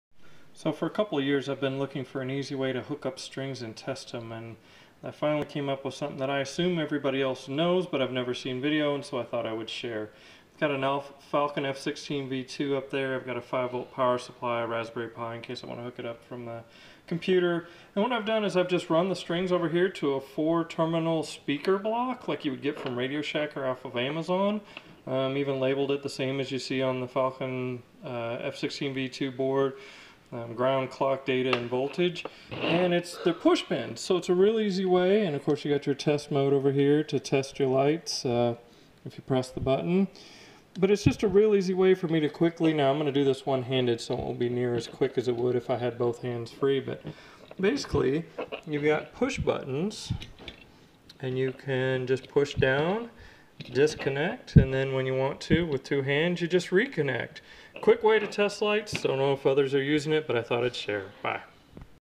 Quick way to hook up and test strings/pixels
Using 4 pin push speaker terminal to make a quick connect for hooking up strings/pixels to test easily.  (Always connect ground first and be sure your controller can handle hot-swapping).